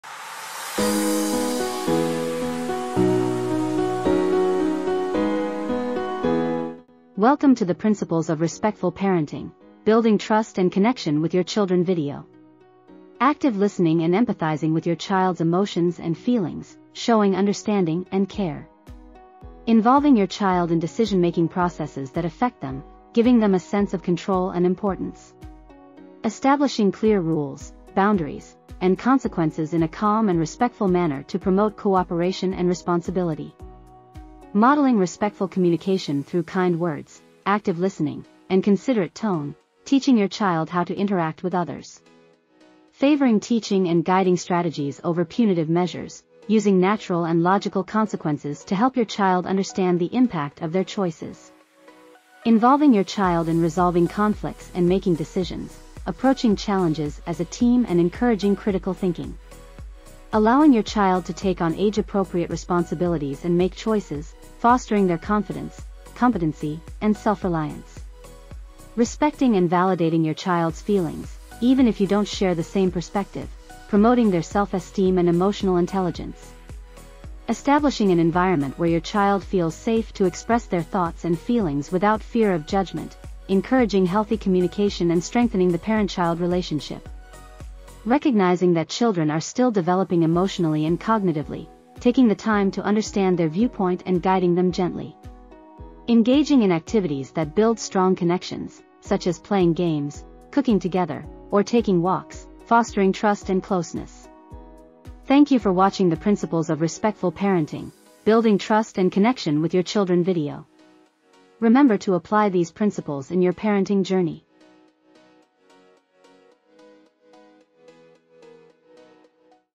Respectful Parenting Building Trust and Connection with Kids
Dive into the transformative world of parenting with respect in our latest video blog, "Respectful Parenting: Building Trust and Connection with Kids."

Discover the power of empathy, active listening, and effective communication in nurturing strong, trusting relationships with your children. Explore practical strategies and heartwarming stories that will help you foster an environment of respect, love, and understanding at home.

Join us on this empowering journey towards building deeper connections with your little ones!

👉 In this video, you'll discover the principles of respectful parenting and provide practical tips for building trust and strong connections with your kids.

Thank you for being part of our parenting community. Together, we can equip our children with the tools they need to thrive and face life's challenges head-on.